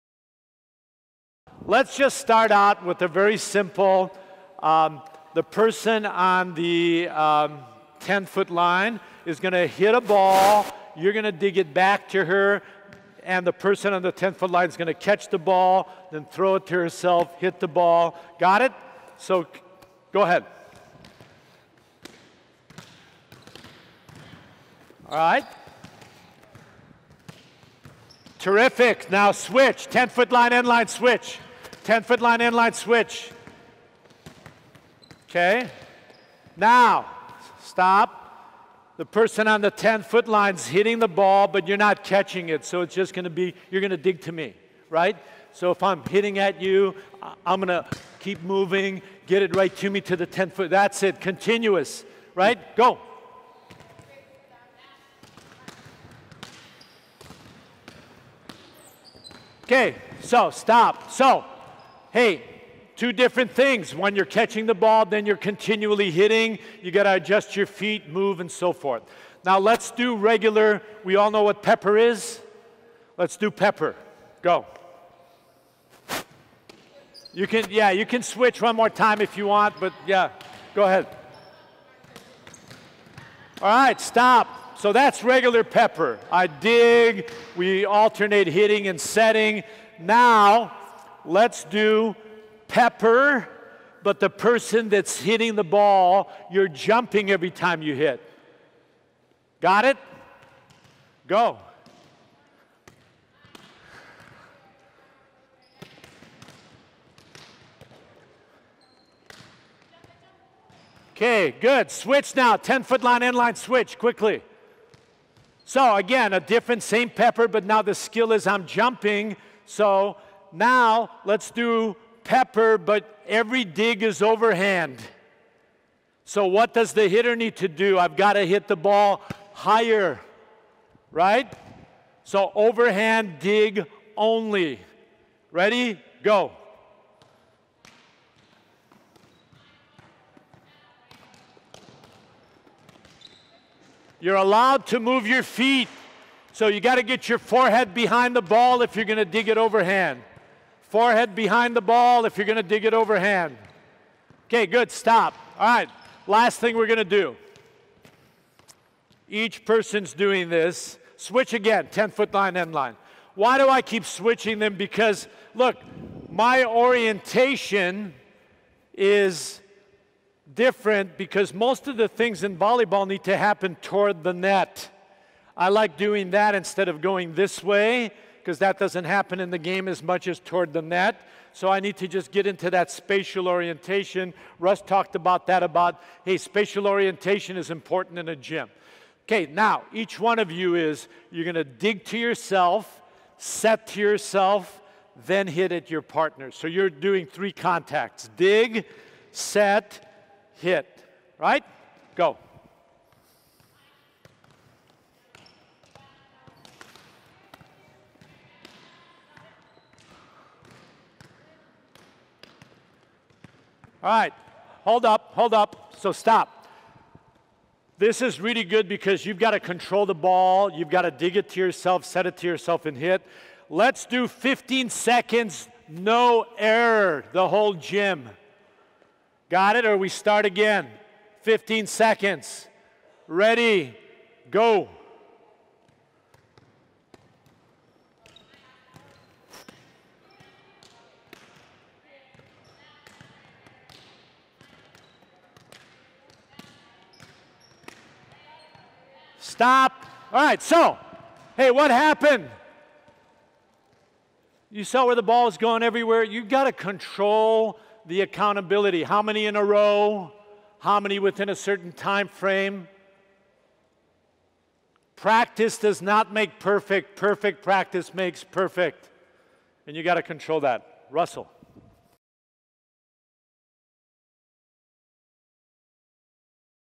AVCA Video Tip of the Week: Ball Control Digging Sequence
Former Team USA and Oregon State Head Coach Terry Liskevych goes over ball control digging sequences.